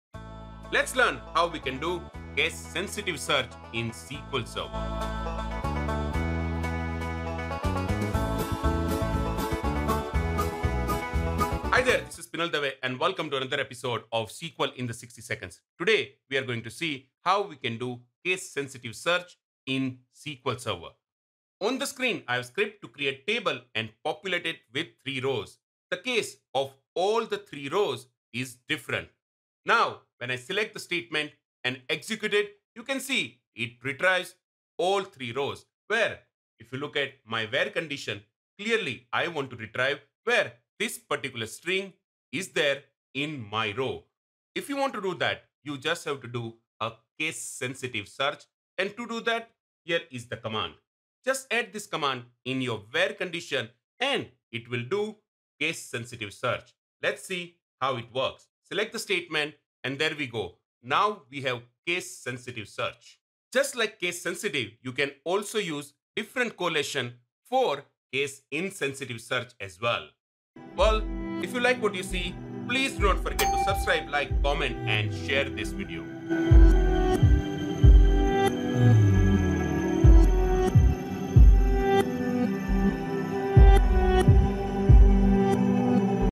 Case Sensitive Search - SQL in Sixty Seconds 158
Let us learn how we can do a Case Sensitive Search in SQL Server.

0:00 Intro
0:11 Real World Story
0:22 Demo - Case Sensitive Search
1:11 Summary